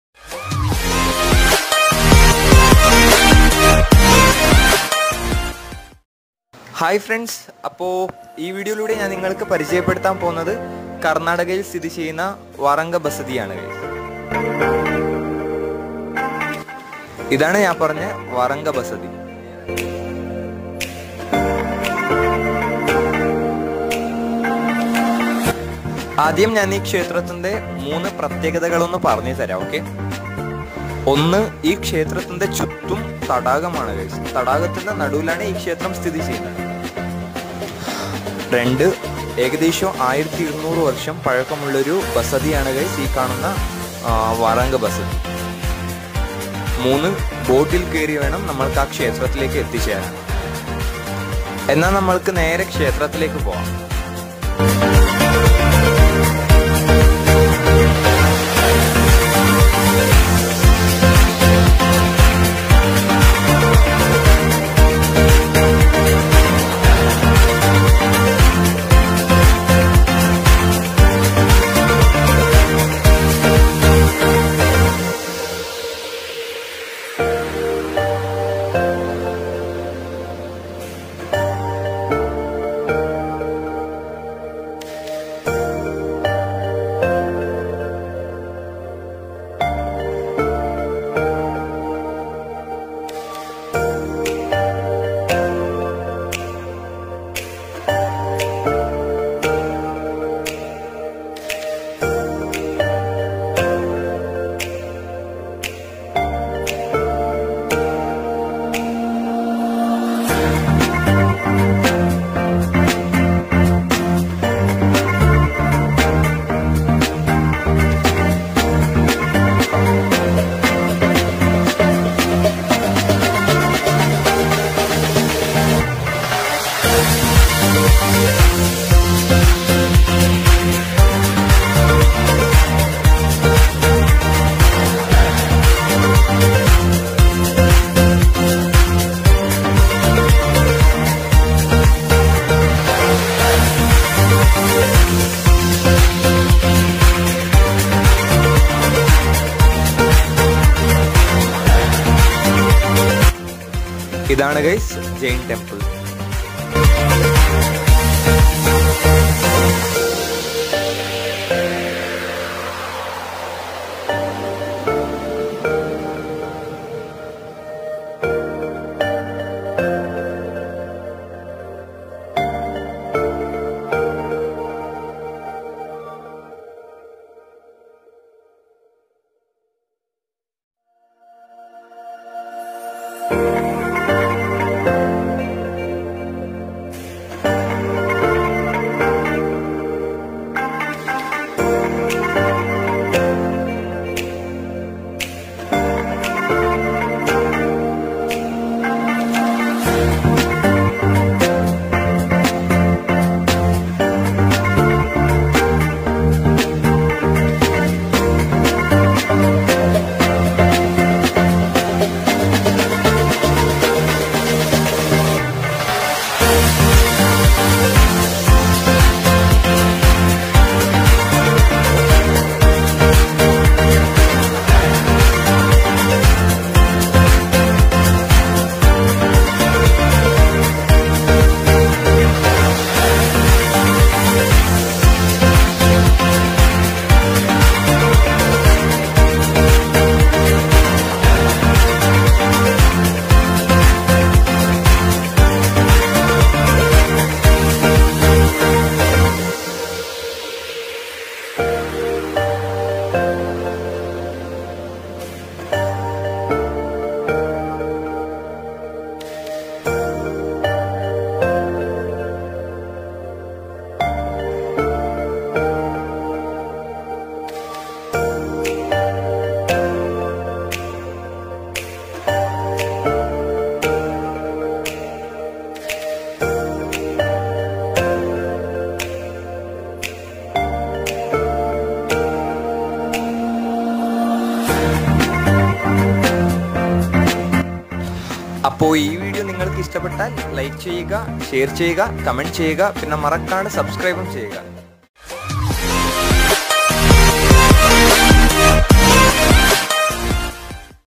Varanga Basadi || Speciality of Varanga_Basadi||Lake Basadi || By Boat||1200 Year's Old
Varanga Basadi
Near agumbe
Karnataka

Speciality of Varanga Basadi

*Lake Basadi
*To the temple,By boat
*Jain Basadi